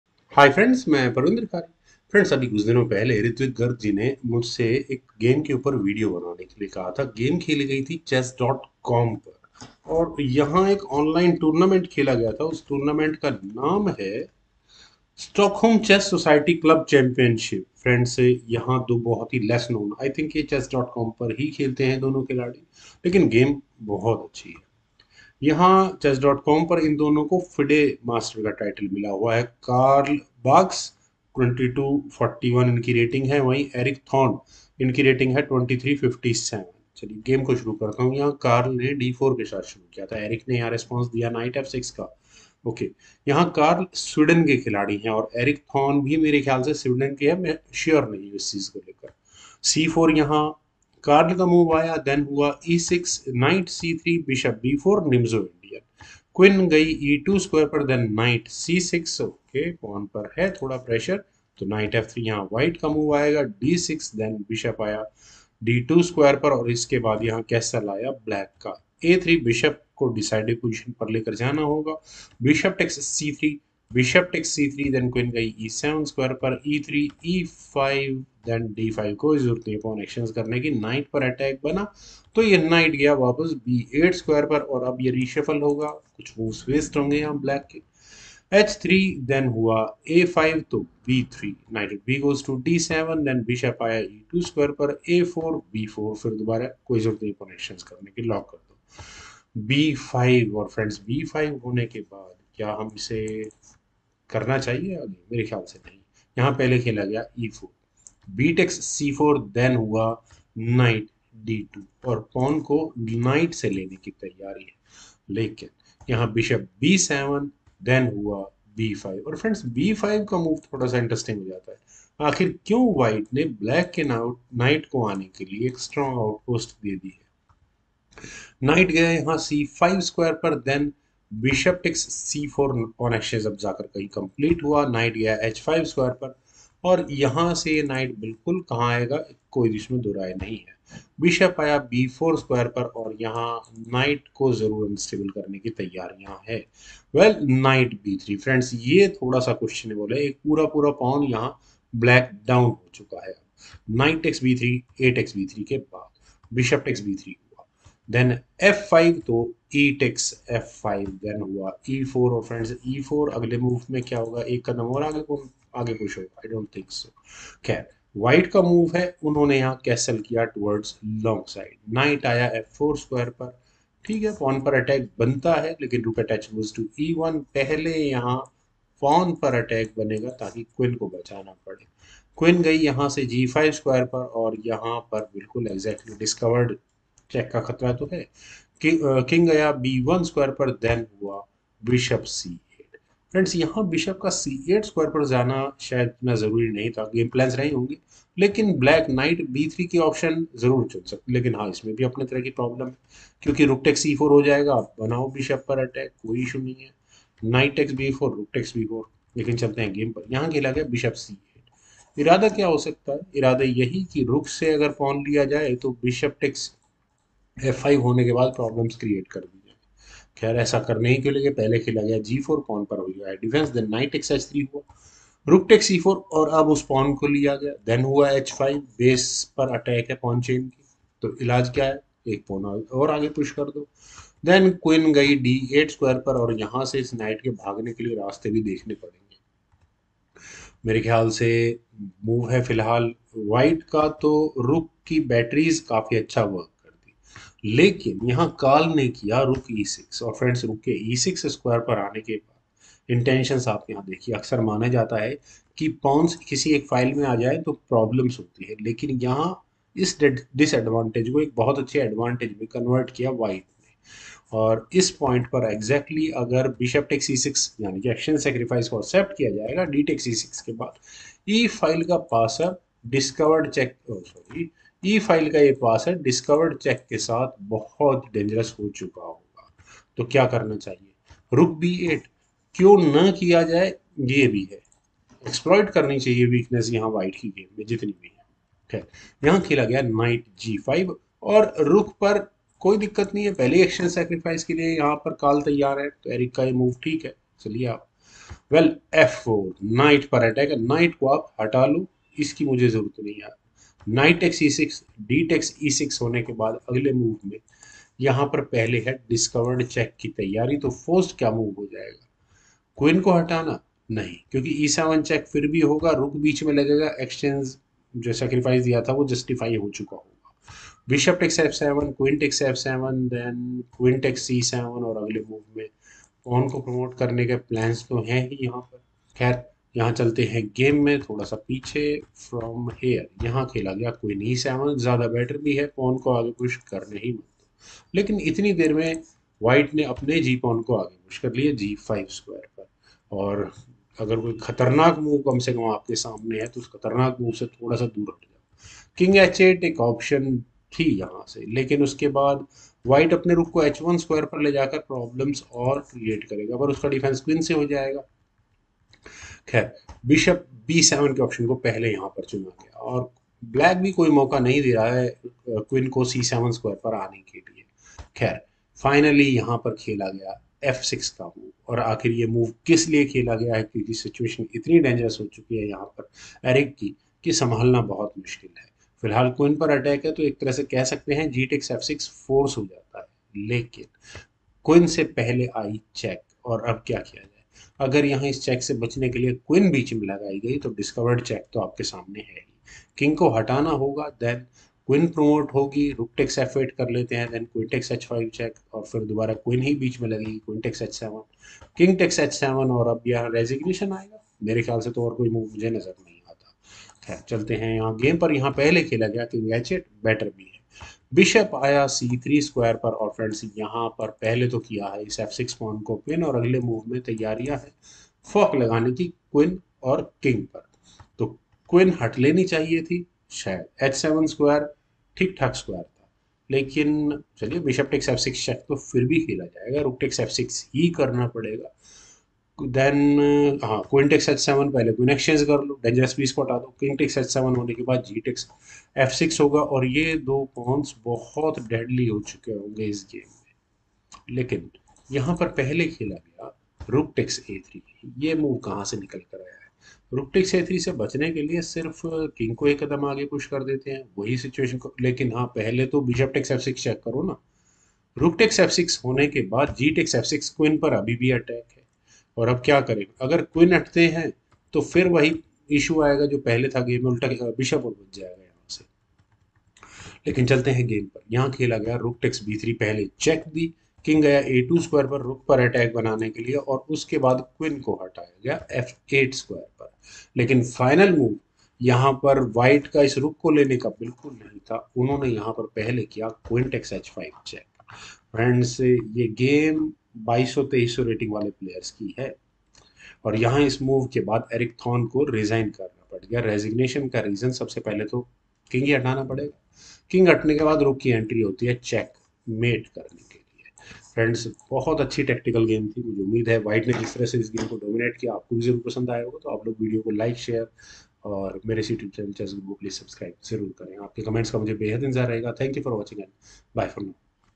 Ordinary Game with Extraordinary Moves
Hi friends and welcome! My self parvinderkhari and this video is about chessanalysis of the game played between Carl Cederstam Barsk vs Eric Thorn Stockholm Chess Society Club Championship
[Event "Stockholm Chess Society Club Championsh"]
[Site "?"]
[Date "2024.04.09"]
[Round "7"]
[White "Carl Cederstam Barsk"]
[Black "Eric Thorn"]
[Result "1-0"]
[ECO "E33"]
[WhiteElo "2241"]
[BlackElo "2357"]
nimzoindian Defense
1. d4 Nf6 2. c4 e6 3. Nc3 Bb4 4. Qc2 Nc6 5. Nf3 d6 6. Bd2 O-O 7. a3 Bxc3 8. Bxc3 Qe7 9. e3 e5 10. d5 Nb8 11. h3 a5 12. b3 Nbd7 13. Be2 a4 14. b4 b5 15. e4 bxc4 16. Nd2 Bb7 17. b5 Nc5 18. Bxc4 Nh5 19. Bb4 Nb3 20. Nxb3 axb3 21. Bxb3 f5 22. exf5 e4 23. O-O-O Nf4 24. Rhe1 Qg5 25. Kb1 Bc8 26. g4 Nxh3 27. Rxe4 h5 28. f3 Qd8 29. Re6 Ng5 30. f4 Nxe6 31. dxe6 Qe7 32. g5 Bb7 33. f6 gxf6 34. Qg6+ Kh8 35. Bc3 Rxa3 36. Bxf6+ Rxf6 37. gxf6 Rxb3+ 38. Ka2 Qf8 39. Qxh5+ 1-0

Download the android game
2048 Real Brain Teaser